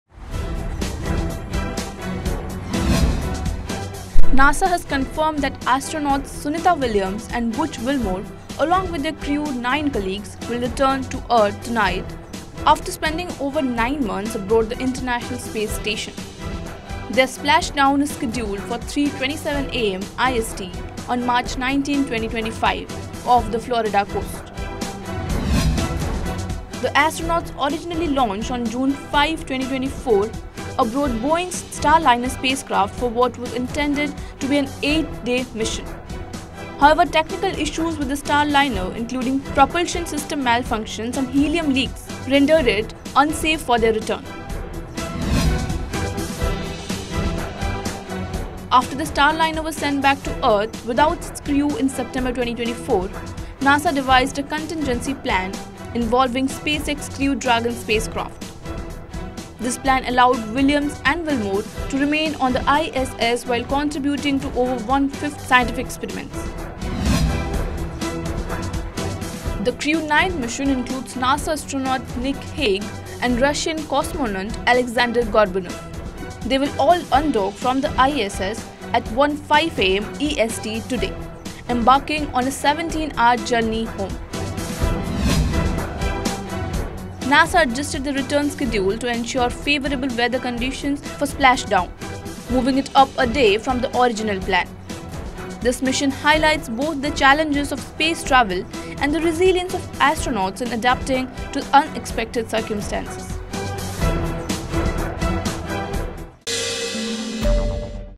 NASA Astronauts Set for Earth Return After Extended MissionStranded in Space for Nine Months
NASA Astronauts Set for Earth Return After Extended Mission
Stranded in Space for Nine Months